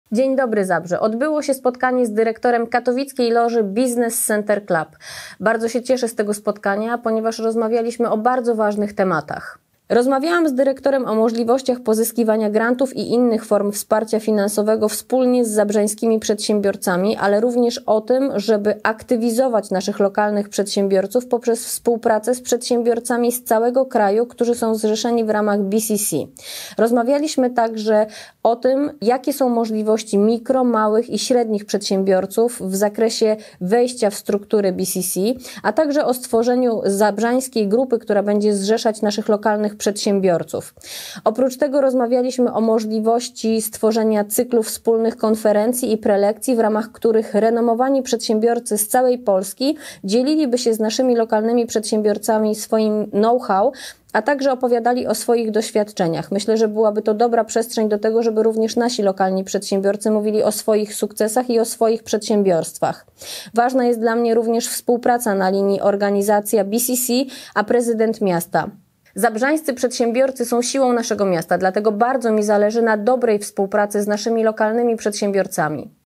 Zabrze przyjaznym miastem dla biznesu
🎥 Krótkie podsumowanie wideo z ostatniego spotkania z Dyrektorem Business Centre Club Katowice, Jackiem Siatkowskim 🤝🏻

Dobra współpraca na rzecz rozwoju zabrzańskich przedsiębiorców to mój priorytet.